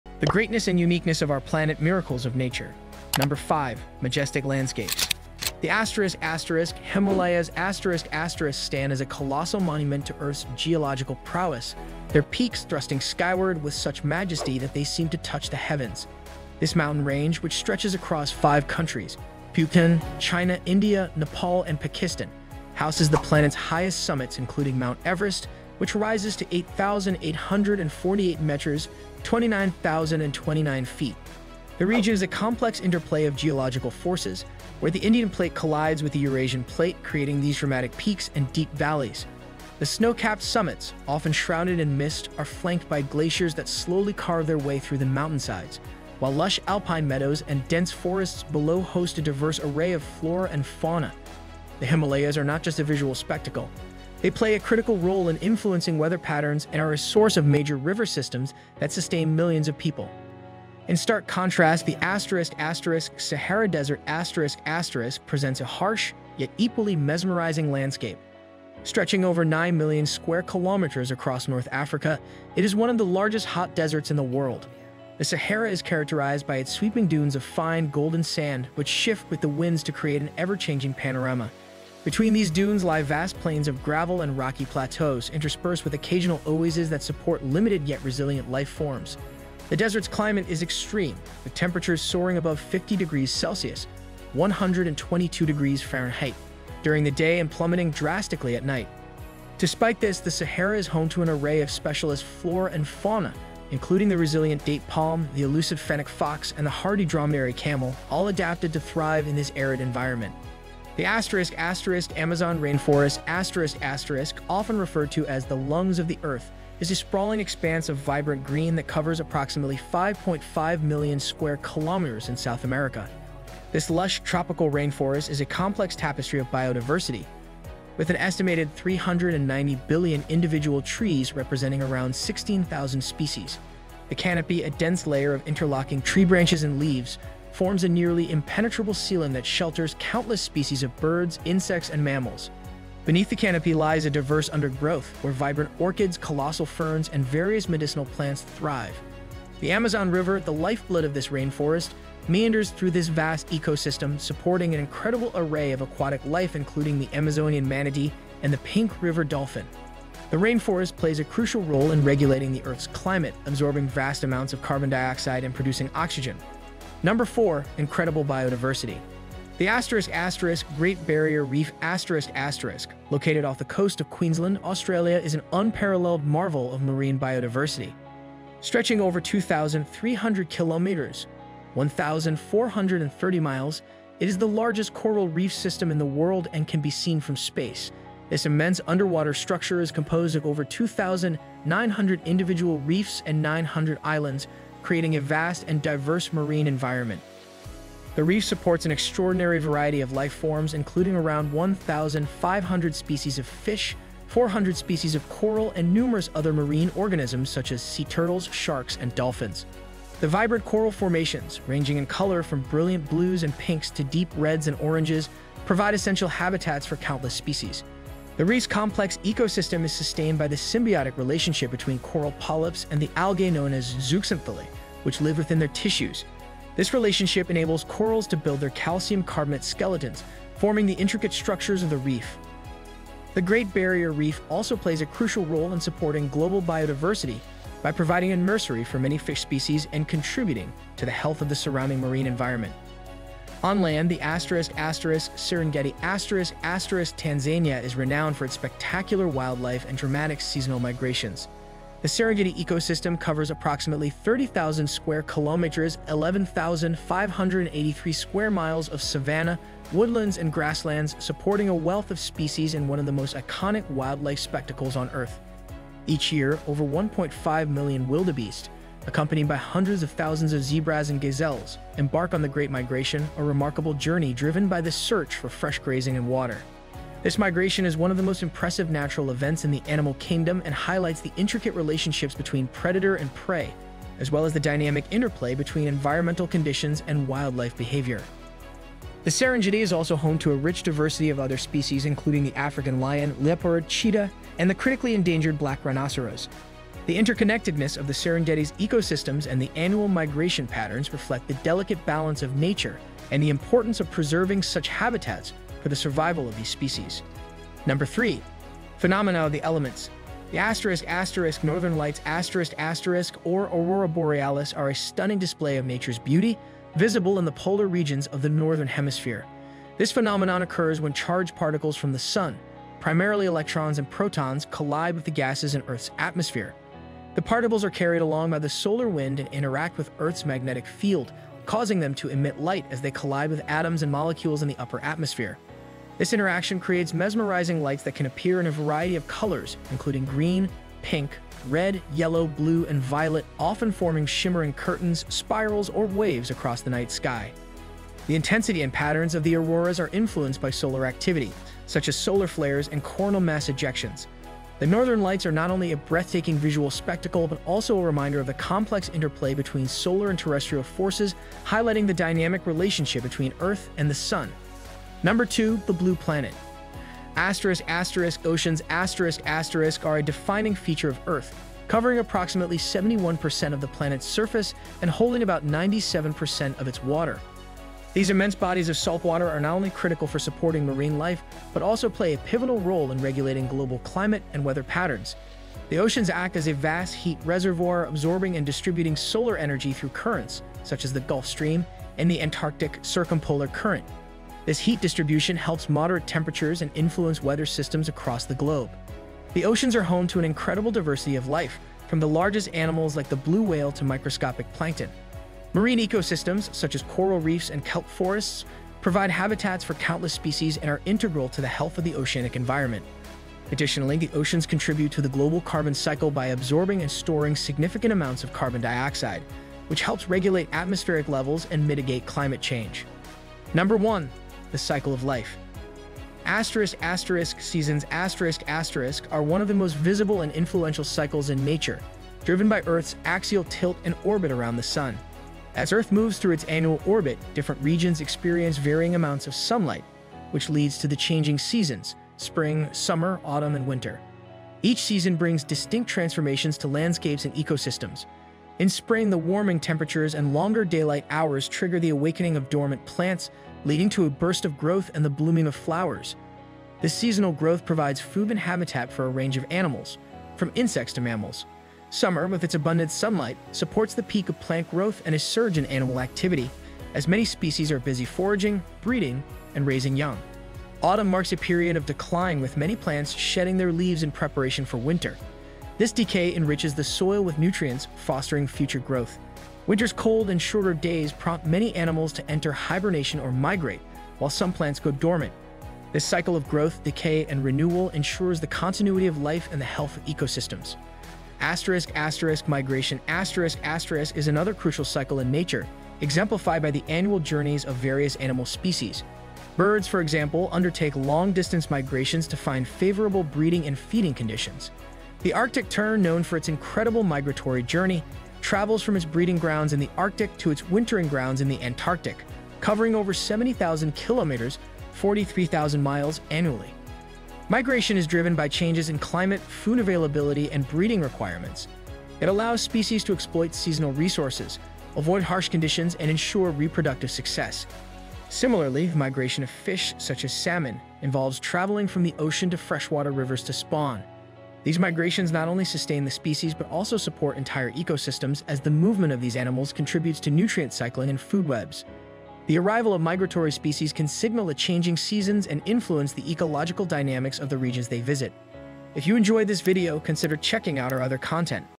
The Greatness And Uniqueness Of Our Planet
In this video, we explore some of the world's most awe-inspiring natural landscapes and phenomena, showcasing their remarkable beauty and significance.

Number 5: Majestic Landscapes

Himalayas: This immense mountain range stretches across five countries and is home to the planet’s highest peaks, including Mount Everest. The Himalayas are a result of the tectonic collision between the Indian and Eurasian plates, featuring snow-capped summits, glaciers, alpine meadows, and dense forests. They play a crucial role in weather patterns and are a source of major rivers.
Sahara Desert: Spanning over 9 million square kilometers, the Sahara is a vast hot desert with shifting golden dunes, rocky plateaus, and occasional oases. Despite its extreme climate, it supports specialized flora and fauna adapted to the harsh conditions.
Amazon Rainforest: Covering approximately 5.5 million square kilometers, this tropical rainforest is incredibly biodiverse, with around 390 billion trees and numerous species. It regulates the Earth’s climate and houses diverse wildlife, including the Amazonian Manatee and Pink River Dolphin.
Number 4: Incredible Biodiversity

Great Barrier Reef: The world’s largest coral reef system stretches over 2,300 kilometers off Queensland, Australia. It supports a vast array of marine life, including 1,500 fish species and 400 coral species, and plays a critical role in marine ecosystems.
Serengeti: Located in Tanzania, the Serengeti is renowned for its rich wildlife and the annual Great Migration of wildebeest, zebras, and gazelles. This migration highlights the interconnectedness of predator-prey relationships and the importance of preserving these ecosystems.
Number 3: Phenomena of the Elements

Northern Lights (Aurora Borealis): This stunning light display occurs in polar regions when charged solar particles interact with Earth’s atmosphere. The lights create vibrant colors and patterns in the night sky, influenced by solar activity.
Number 2: The Blue Planet

Oceans: Covering about 71% of Earth’s surface, oceans are vital for marine life and climate regulation. They act as heat reservoirs, support diverse ecosystems, and play a key role in the global carbon cycle.
Number 1: The Cycle of Life

Seasons: Driven by Earth’s axial tilt and orbit, the changing seasons bring transformations in landscapes and ecosystems. Each season impacts plant and animal life, contributing to the cycle of growth, decay, and renewal.
Migration: Animal migrations, such as those of the Arctic Tern and salmon, are driven by environmental changes and are crucial for species survival and ecosystem health.